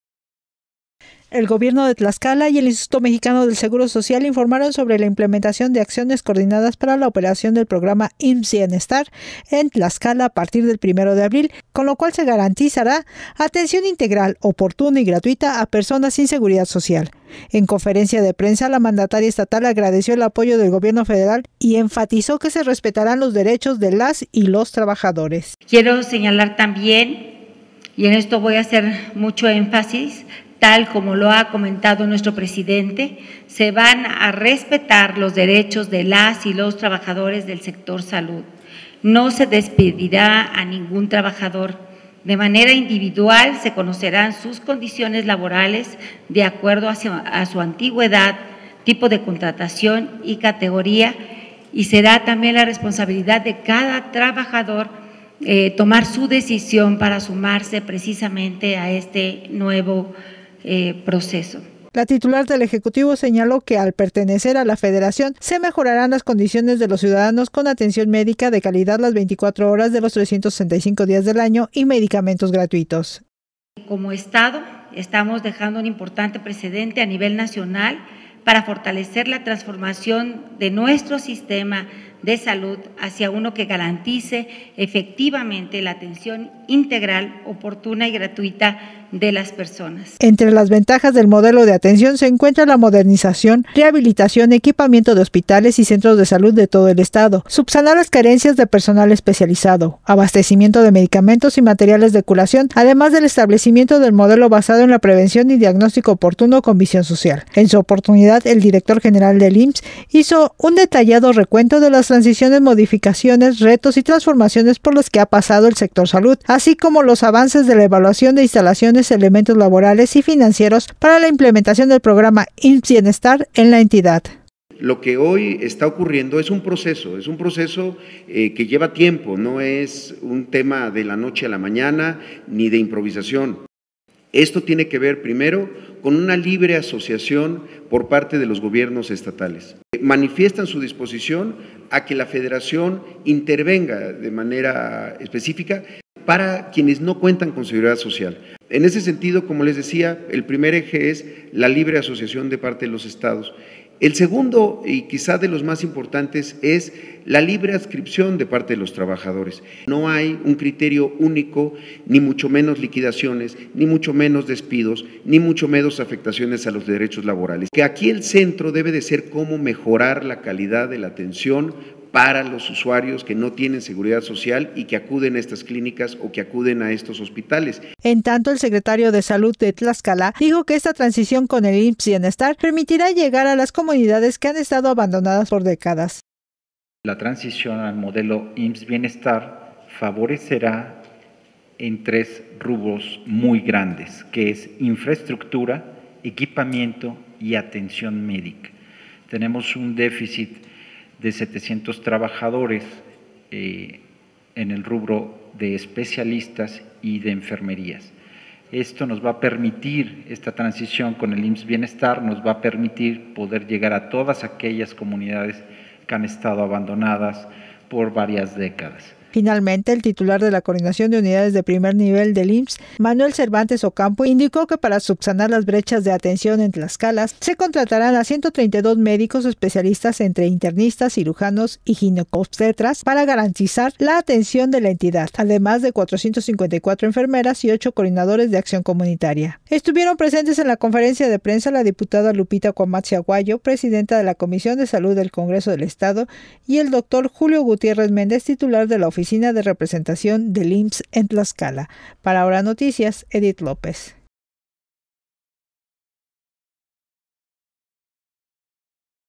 Gobierno de Tlaxcala e IMSS coordinan acciones para transferencia al programa IMSS- Bienestar
de salud al programa IMSS- Bienestar
La gobernadora de Tlaxcala, señaló que la operatividad de las unidades médicas será más eficiente y se mejorará la atención a la población que menos tiene.